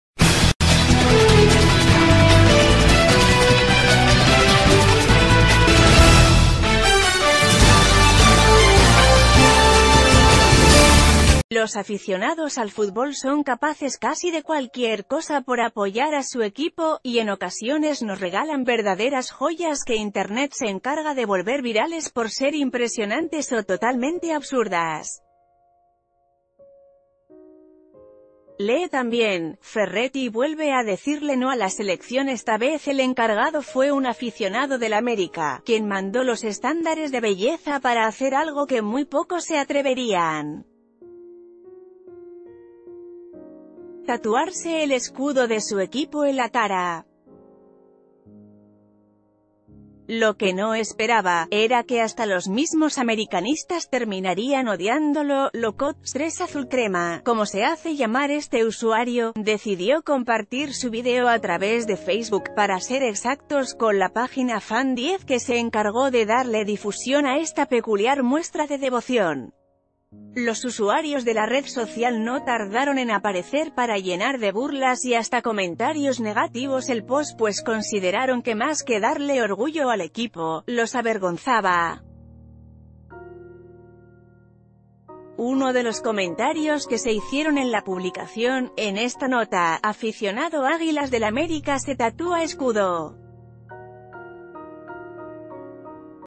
Aficionado del América se tatúa el escudo en la cara y es blanco de burlas
Los aficionados al futbol son capaces casi de cualquier cosa por apoyar a su equipo, y en ocasiones nos regalan verdaderas joyas que internet se encarga de volver virales por ser impresionantes o totalmente absurdas. Lee también: Ferretti vuelve a decirle no a la selecciónEsta vez el encargado fue u...